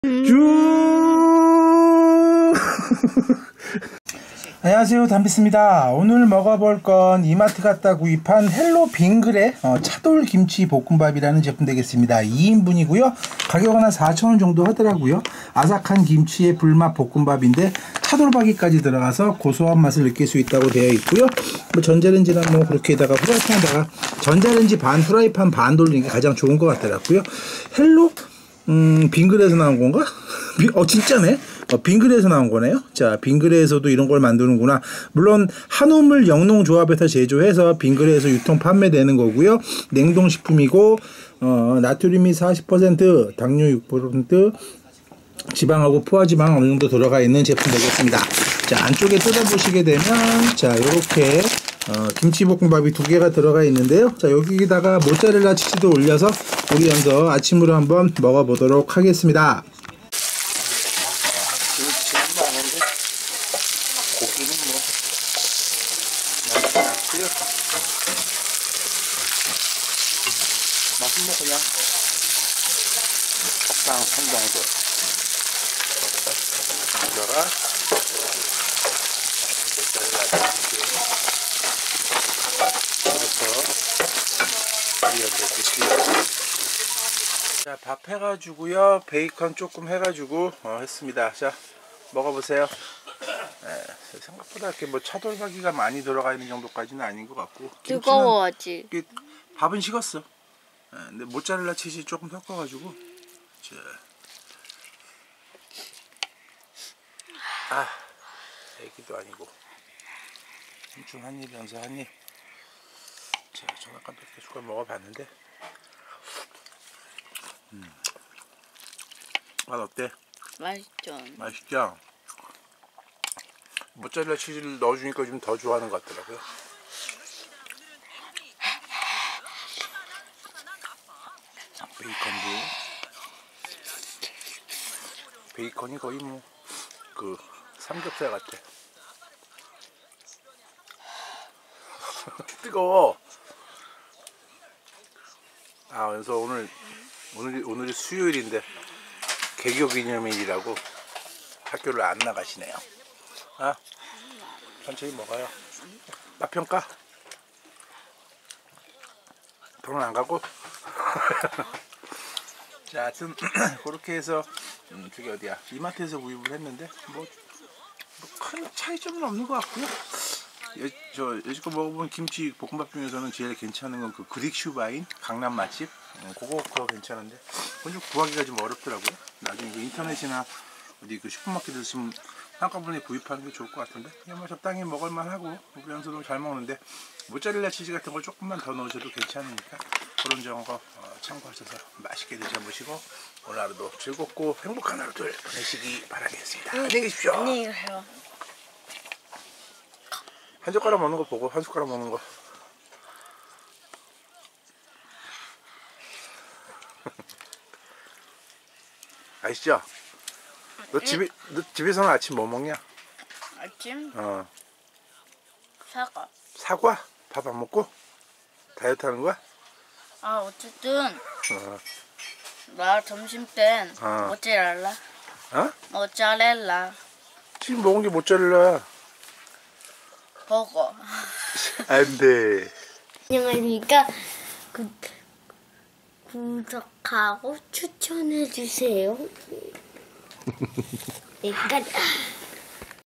차돌김치 볶음밥, 이마트 헬로 빙그레 김치볶음밥 시식기
대형마트에서 4천원에 구입한 2인분인데, 적당히 먹을만...